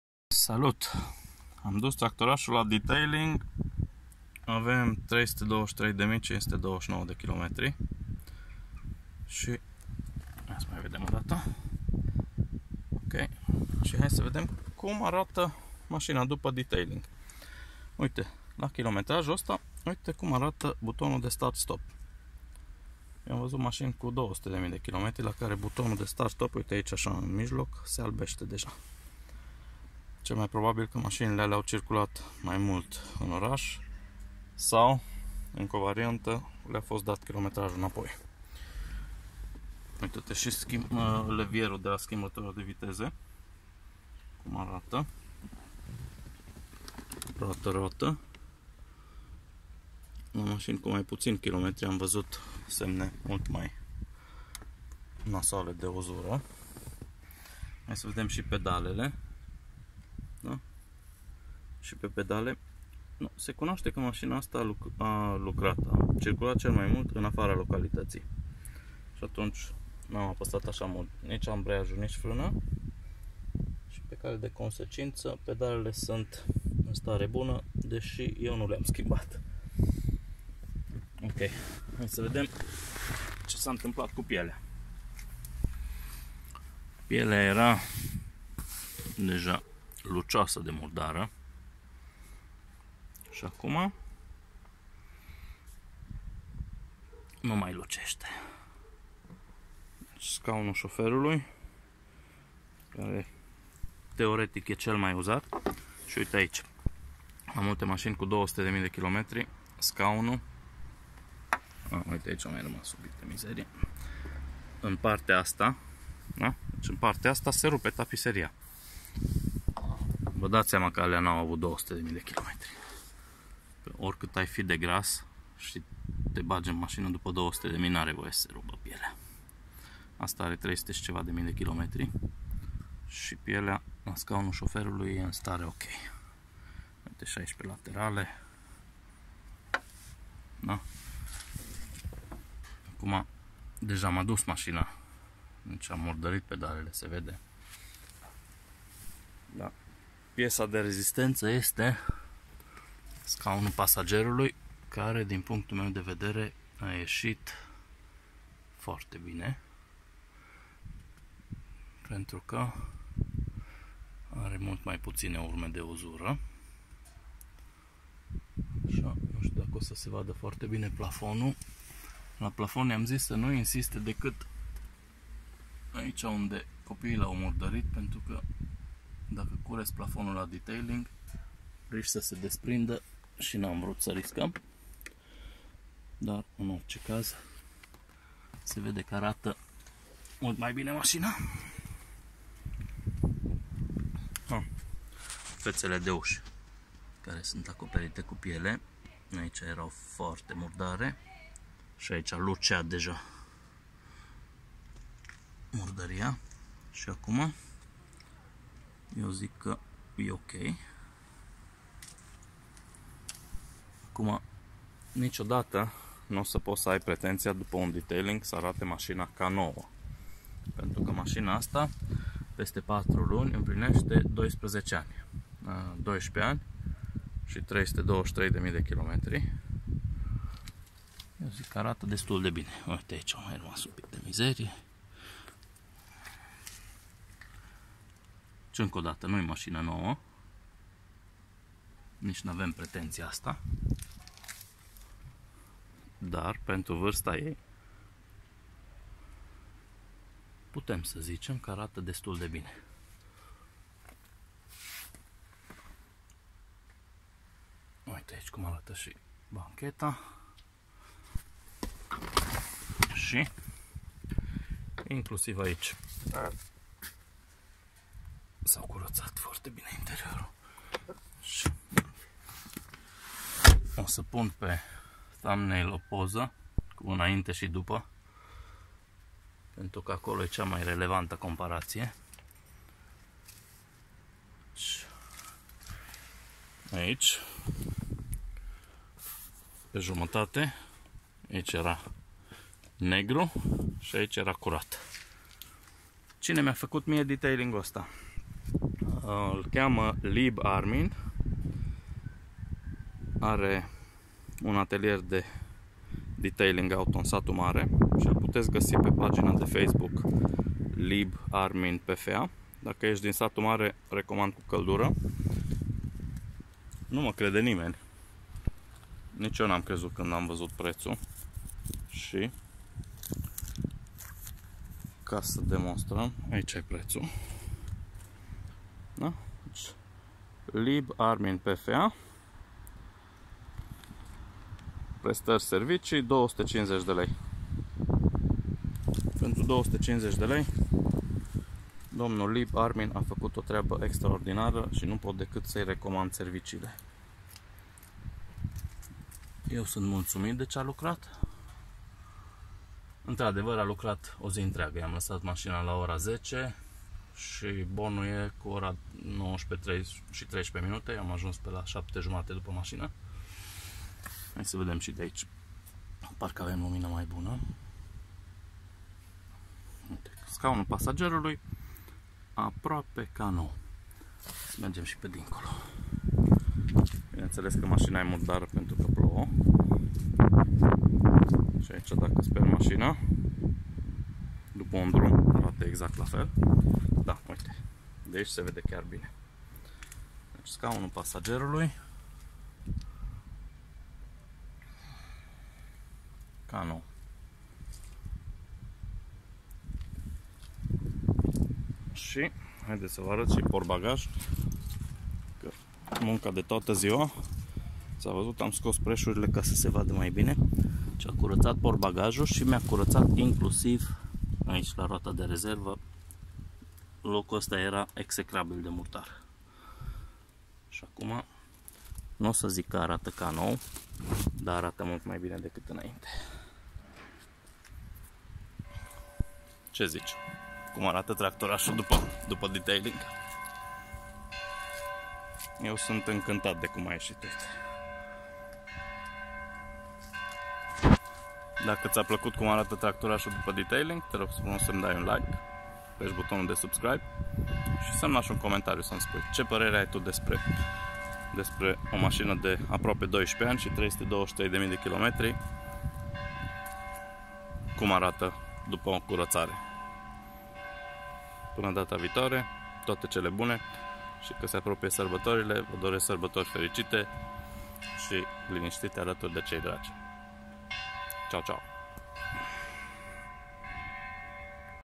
Am dus tractorasul la detailing si uite ce-a iesit. E un BMW 520d E60 LCI de aproape 12 ani
Daca vrei sa vezi pozele cu masina inainte si dupa detailing intra pe pagina de Facebook : Lieb Armin PFA

Tractorasul avea mare nevoie de un detailing auto de o bucata buna de vreme. Lieb Armin din Satu Mare a facut o treaba excelenta si interiorul tractorasului arata foarte bine. Ba mai mult, a curatat si dezinfectat chiar si gurile de ventilatie.